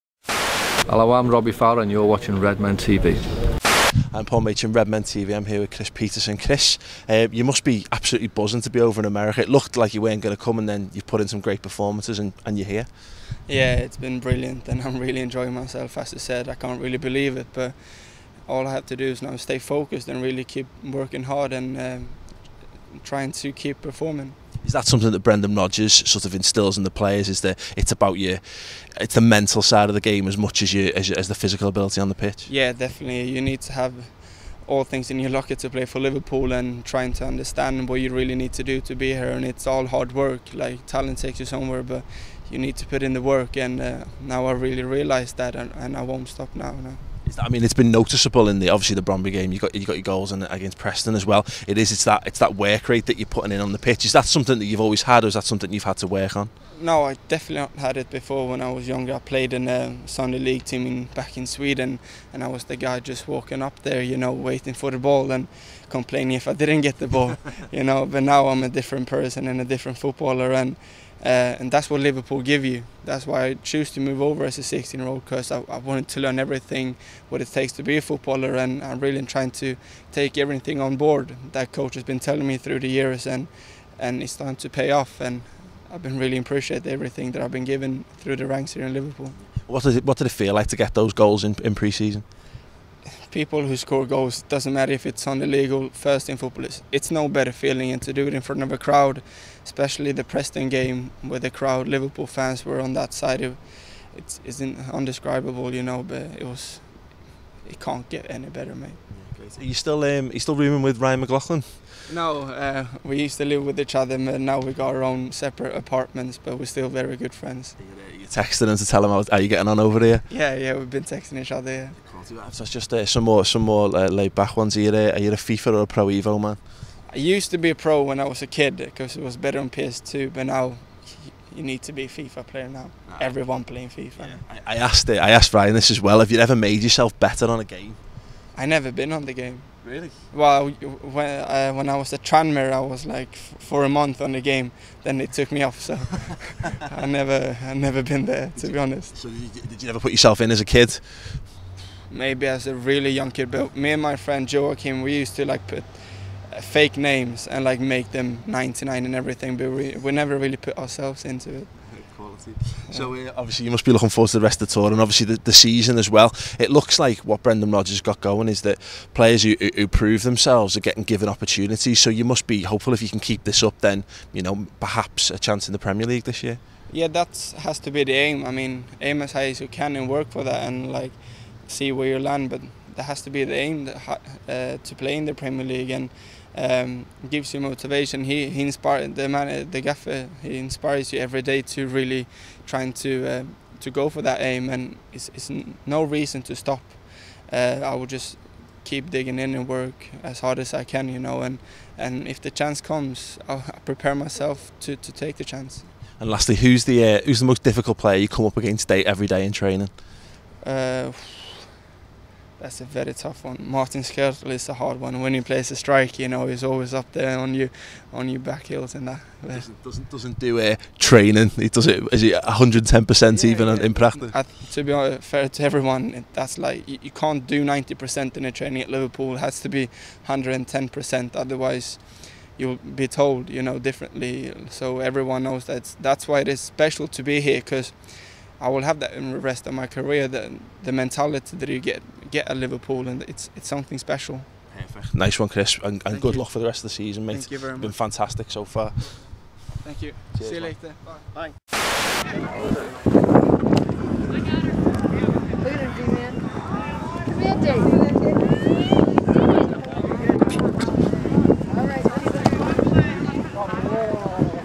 "Talent Takes You Somewhere, You Need To Work Hard" | Kris Peterson Interview EXCLUSIVE
Kristoffer Peterson, arguably the stand out performer of Liverpool's pre-season so far talks exclusively to Redmen TV about the dedication required to play at Liverpool, scoring goals and the age old FIFA/Pro Evo question...

kris Peterson goals highlights interview discussion interview brondby preston